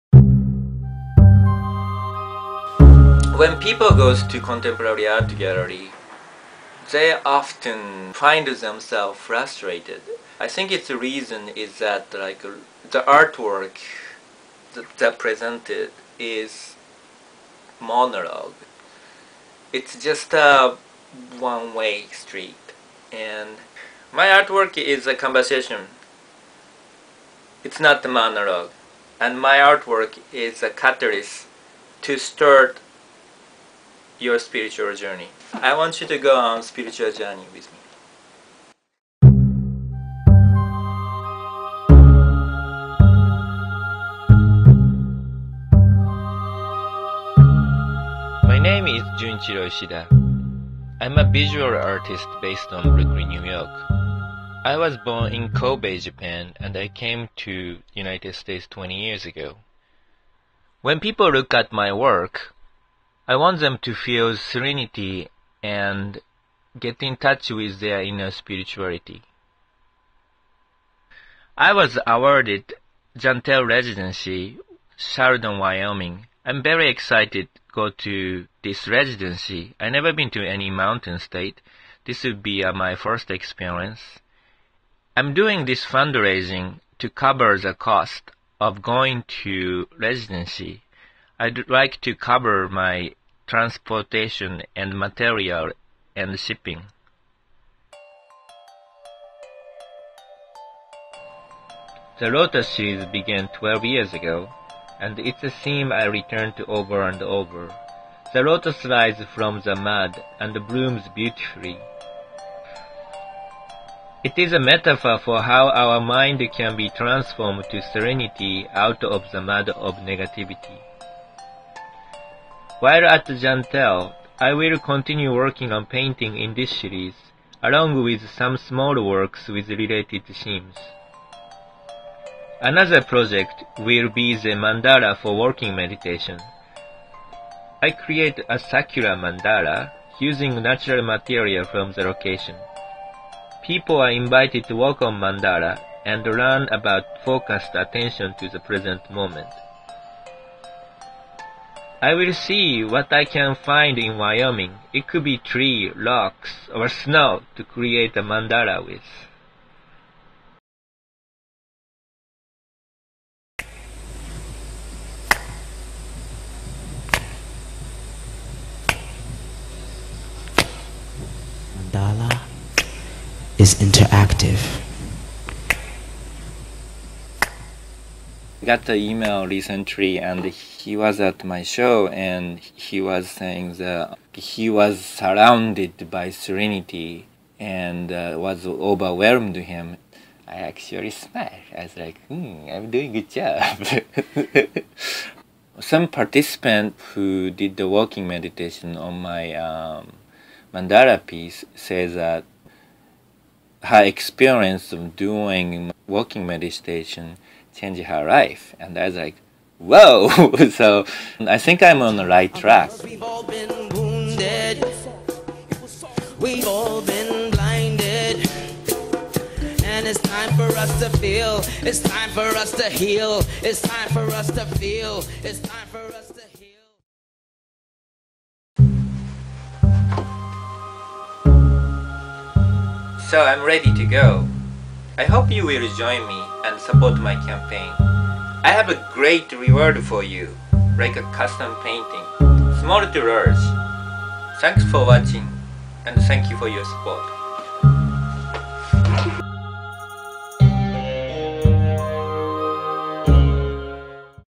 Jun'ichiro Ishida: A Lotus for You
Artist, Jun Ishida, talking about his art, his project at Jentel art residency, and his indiegogo campaign.